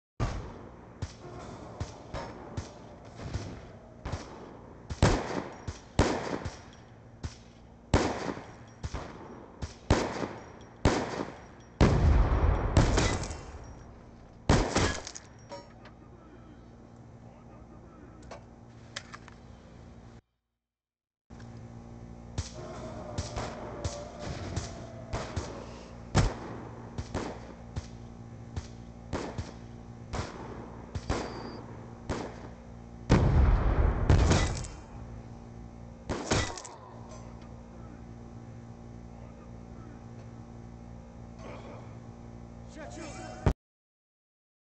How to get headshots with style.
This is how I earn Typhoon camo.